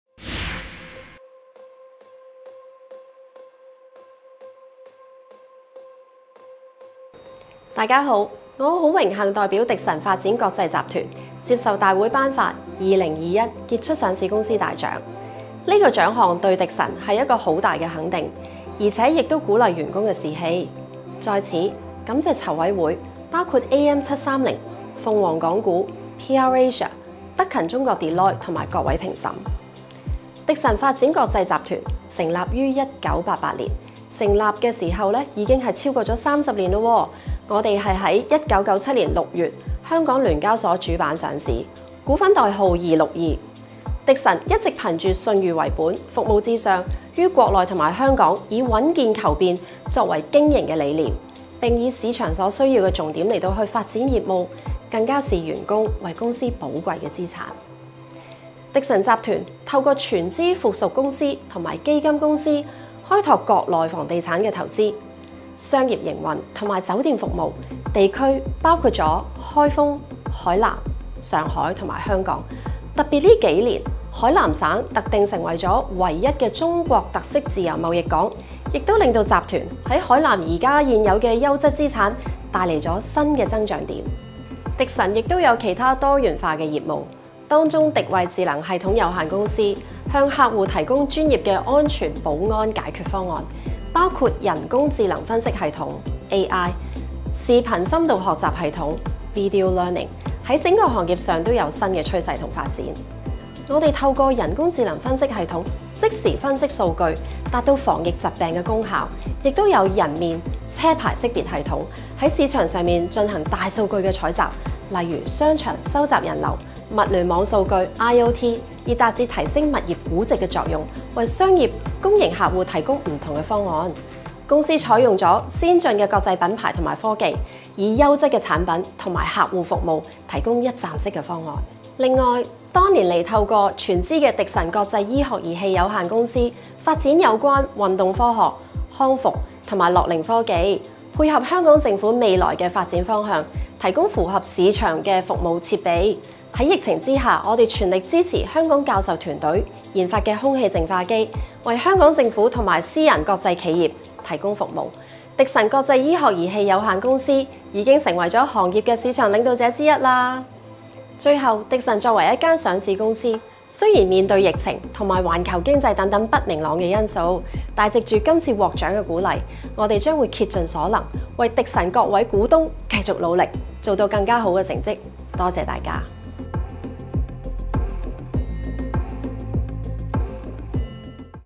【傑出上巿公司大獎2021】迪臣發展國際集團(00262) 信譽為本服務至上 多元業務貢獻社會
迪臣發展國際集團成立於1988年，並於1997年6月香港聯交所主板上市，一直憑著“信譽為本、服務至上”的精神，於國內及香港以穩健求變作為經營理念，並以市場所需為重點發展業務，更視員工為寶貴資產，集團一直致力於吸納及留住業務有關之專才，人員流失率一直低於市場。

bmgproductionmusic